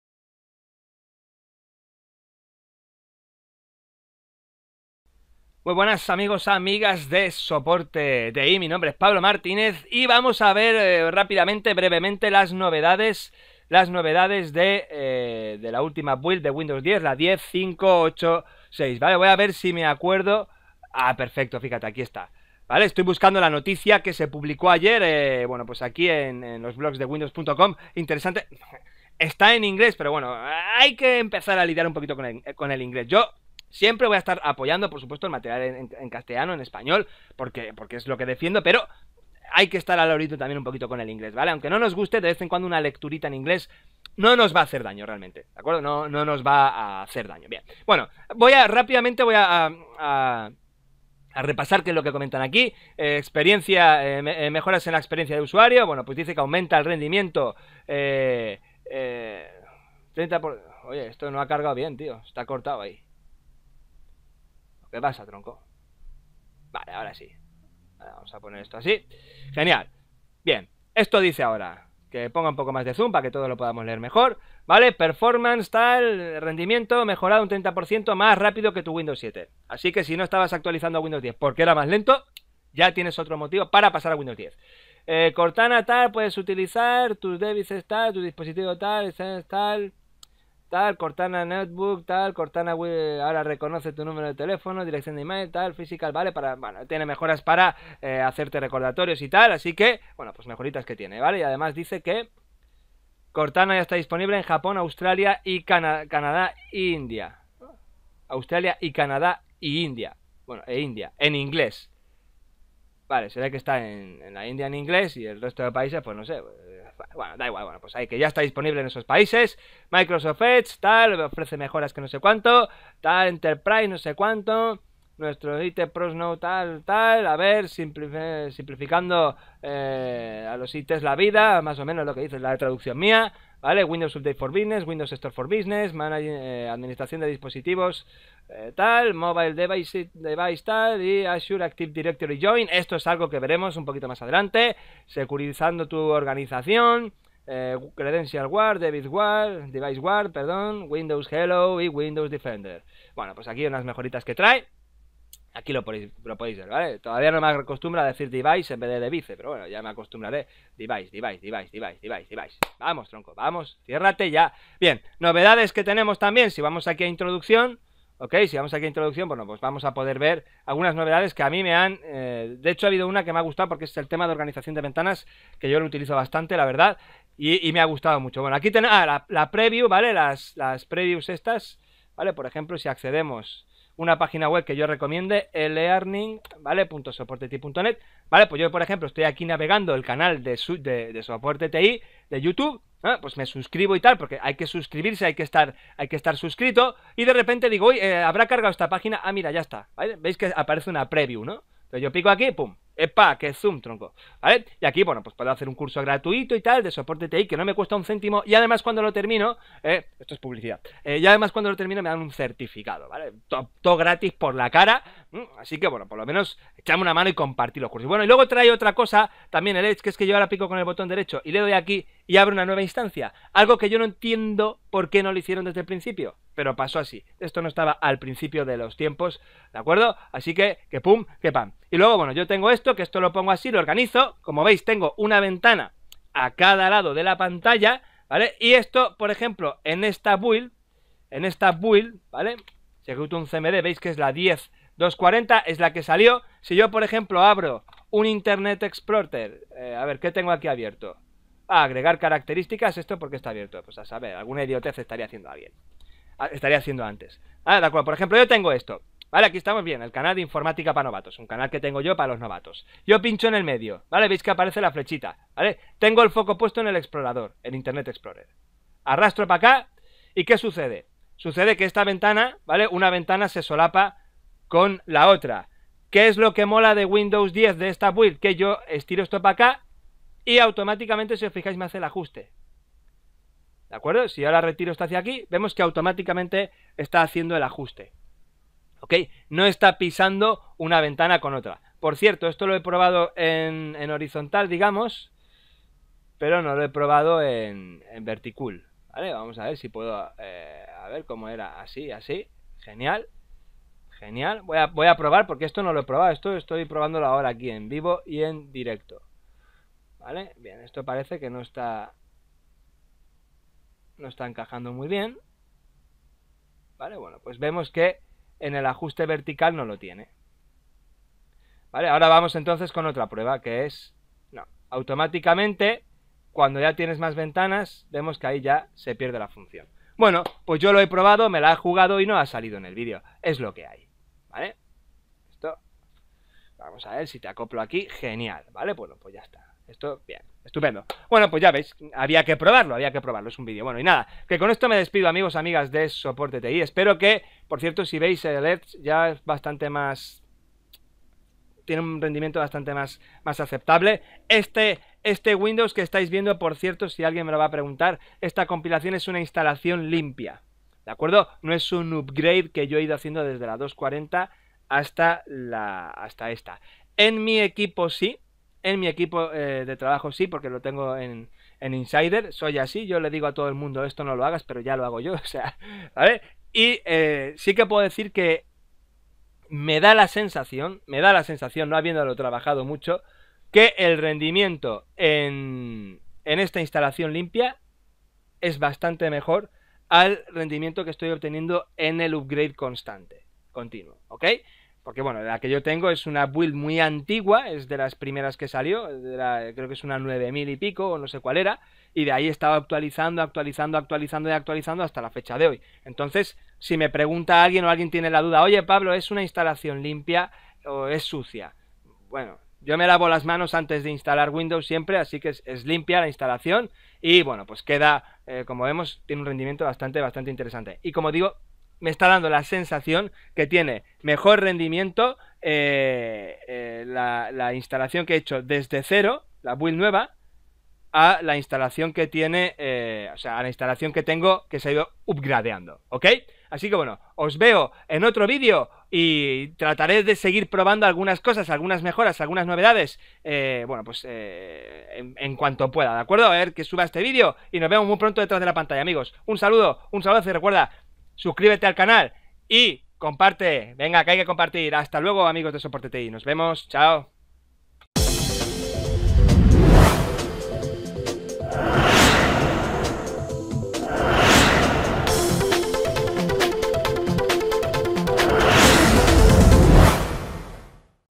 Windows 10 . Algunas novedades de la Build 10586
Ayer día 12 salía la Build 10586 para todo el mundo y tras 12 horas de descargar la ISO aquí tenéis el vídeo con algunas novedades interesantes sobre la Build... Por cierto, devir que estoy trabajando con Edge desde que la tengo en Insider y ha mejorado muchísimo!!